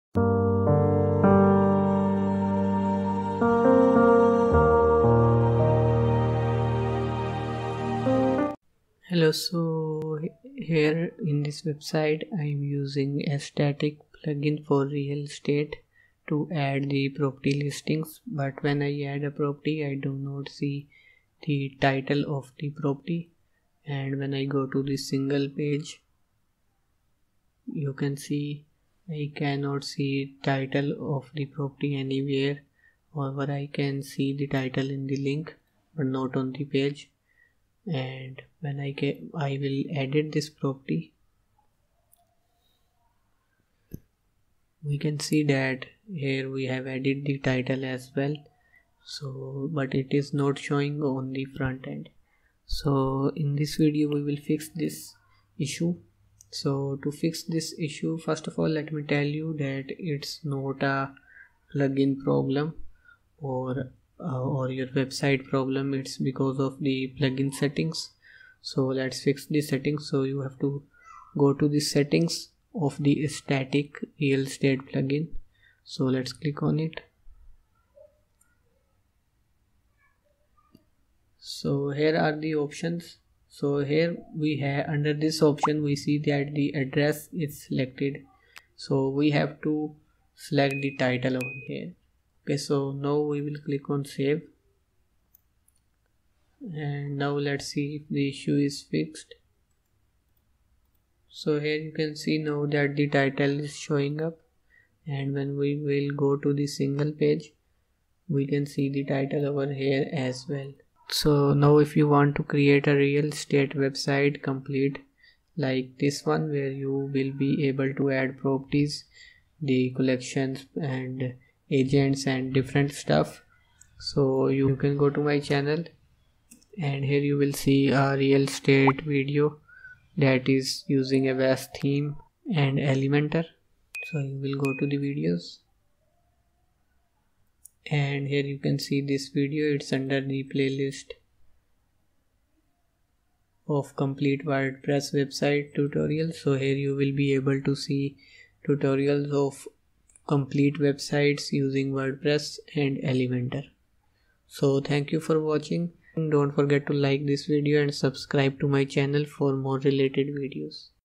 Title Not Showing in Estatik Real Estate WordPress Plugin. FIXED!
Title Not Showing in Estatik Real Estate WordPress Plugin. FIXED! | How to show Title in Estatik Plugin